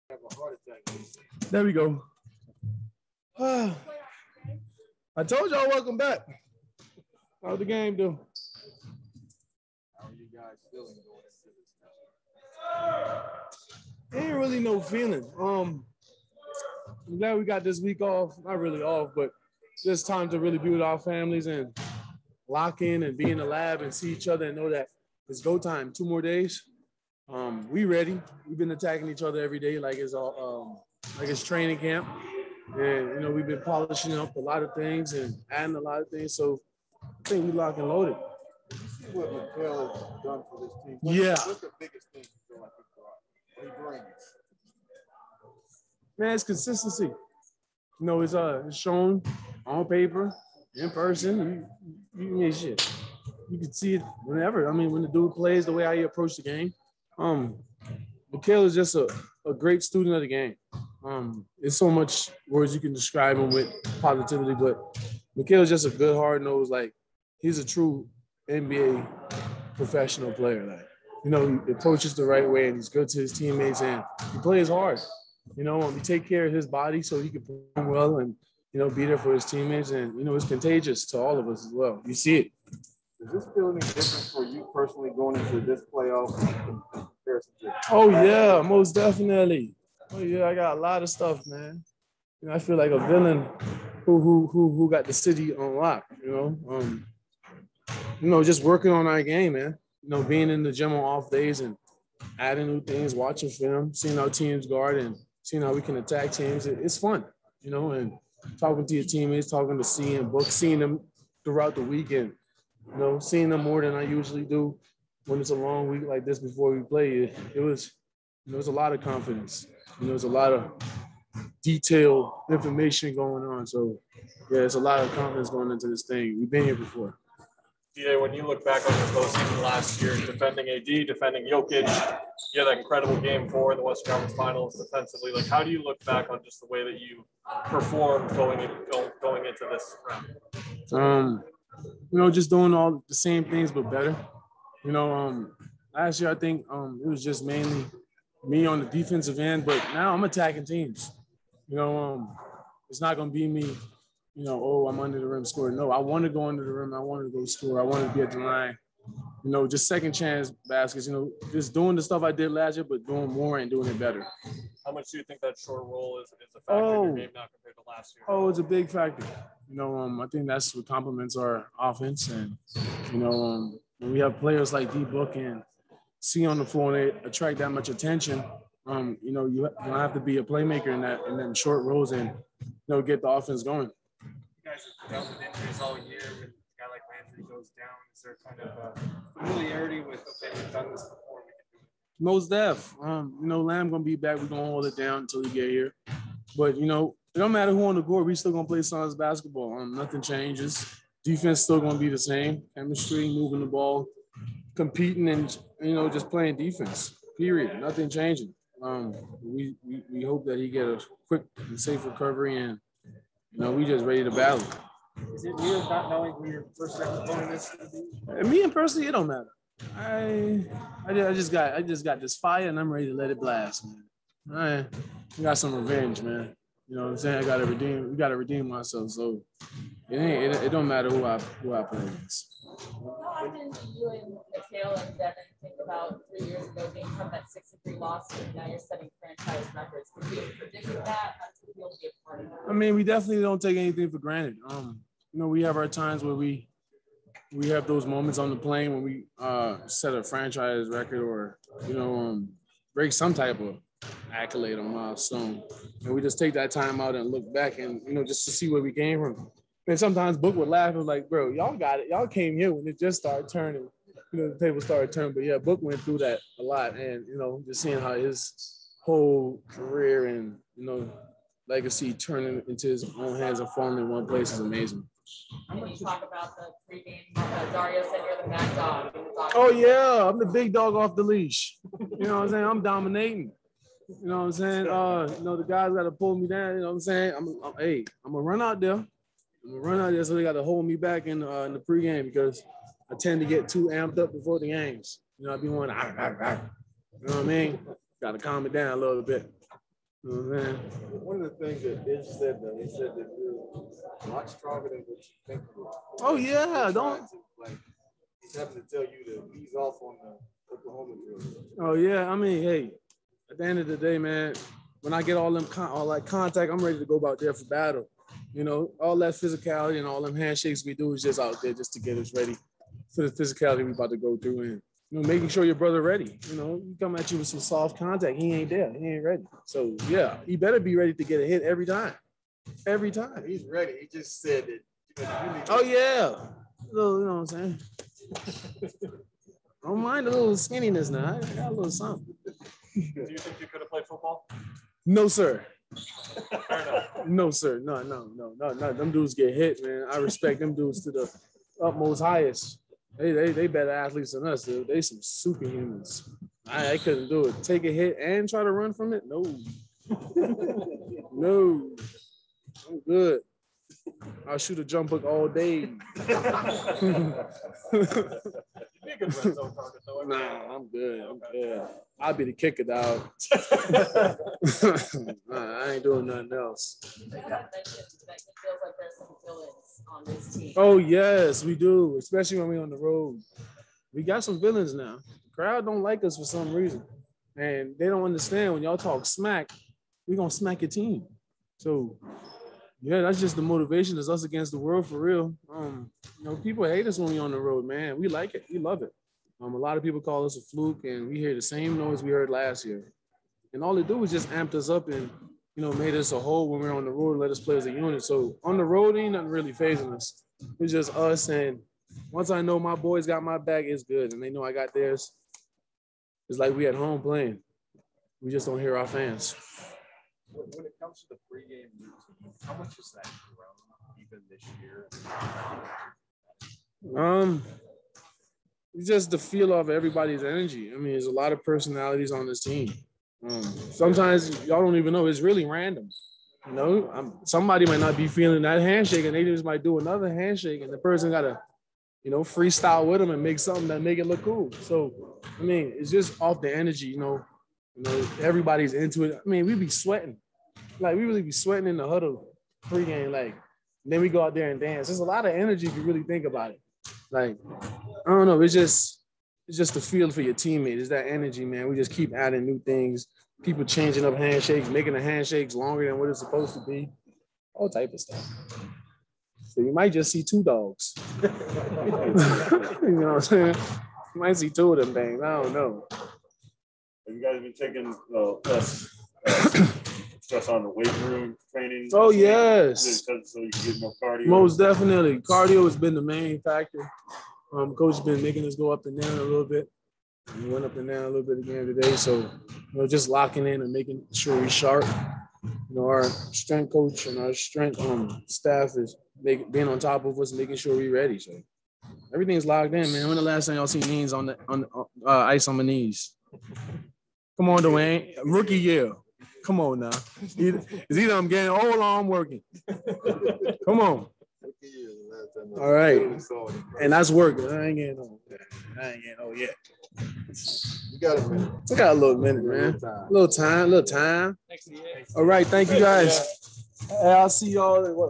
Deandre Ayton practice report; Suns awaits their rival for Game 1 of playoffs
Hey there!

VIDEO SUNS